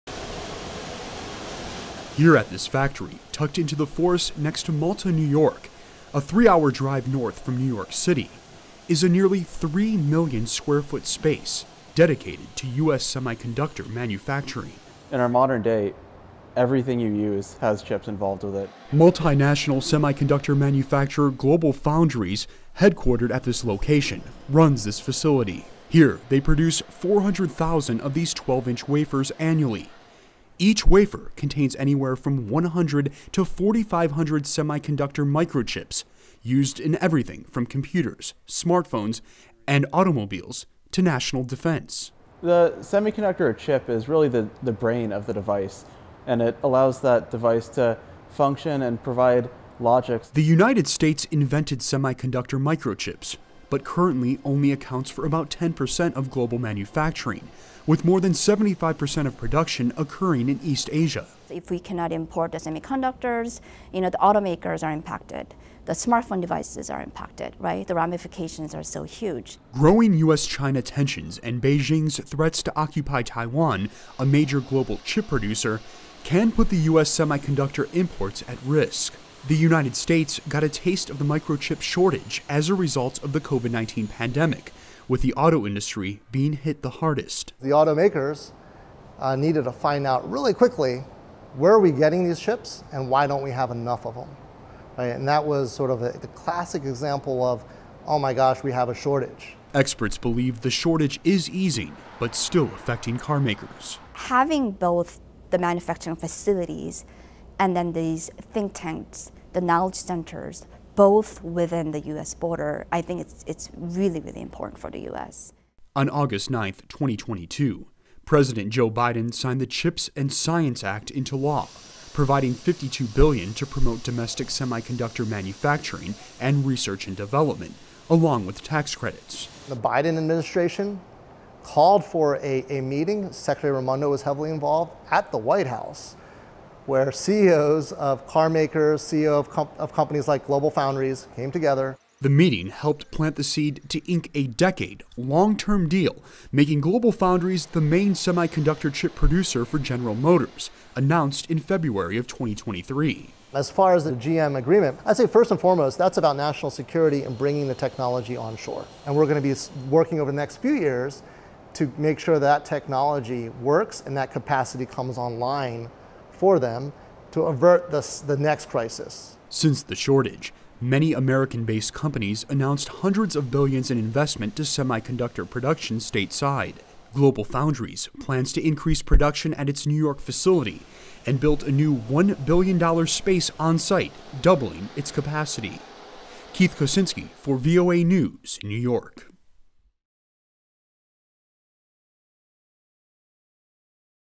US Semiconductor Manufacturing Expected to Ramp Up With New Deal | VOANews
A global shortage of semiconductor chips in the automotive industry starting in 2020 has motivated many countries to increase their domestic manufacturing. The United States has allocated more than $50 billion to promote semiconductor production and research stateside as the global need for the chips is expected to double over the next decade. Keith Kocinski has more from New York. Camera: Keith Kocinski and Rendy Wicaksana.